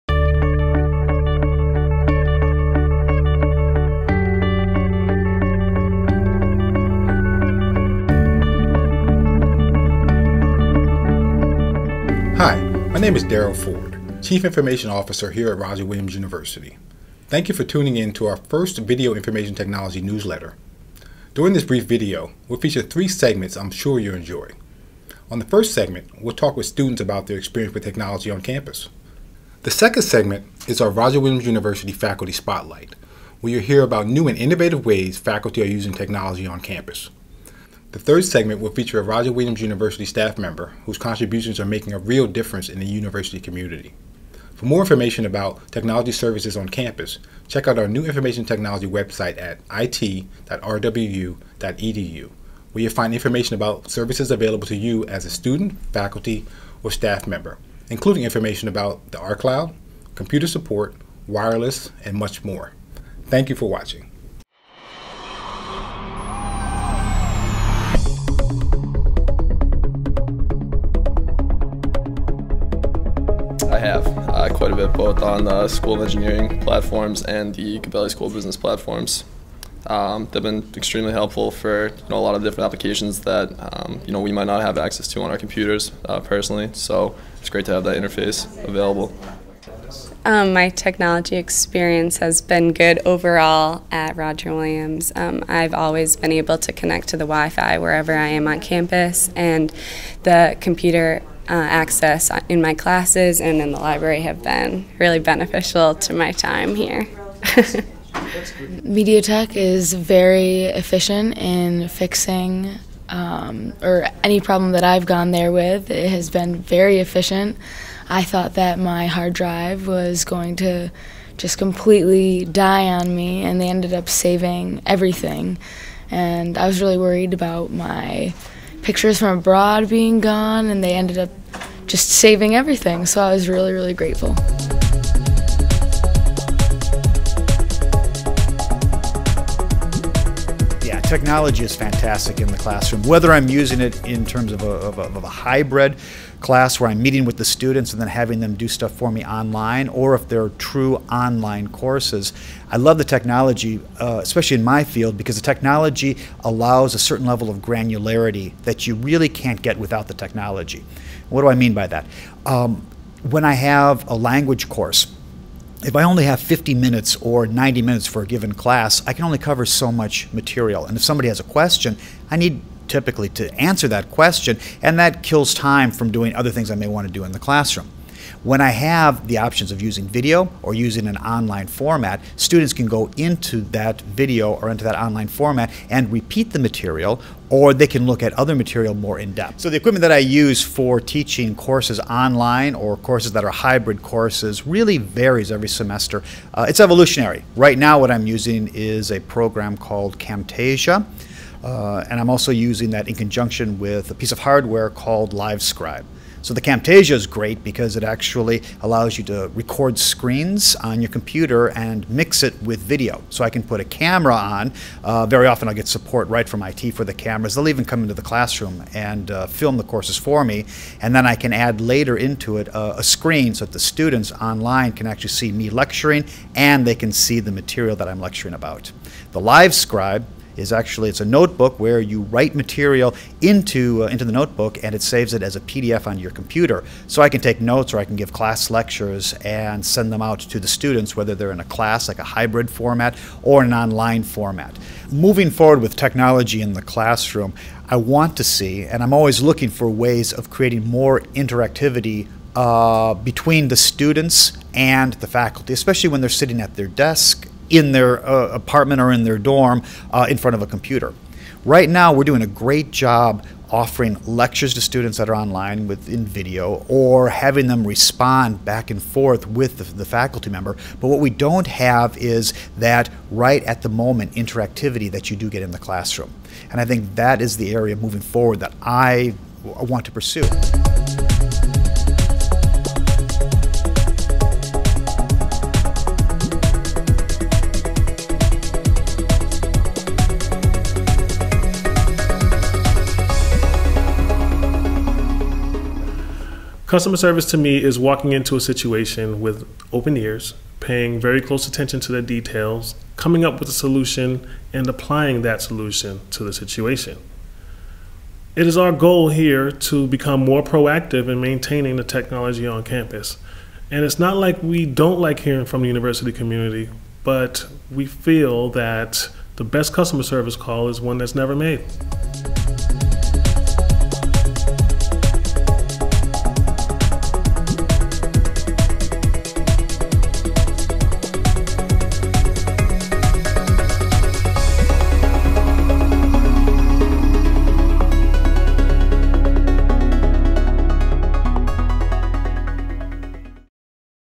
I.T. Newsletter Fall 2015
Featured clips:
Student Spotlight
Faculty Spotlight, (Anthony Hollingsworth)
I.T. Service Award Winner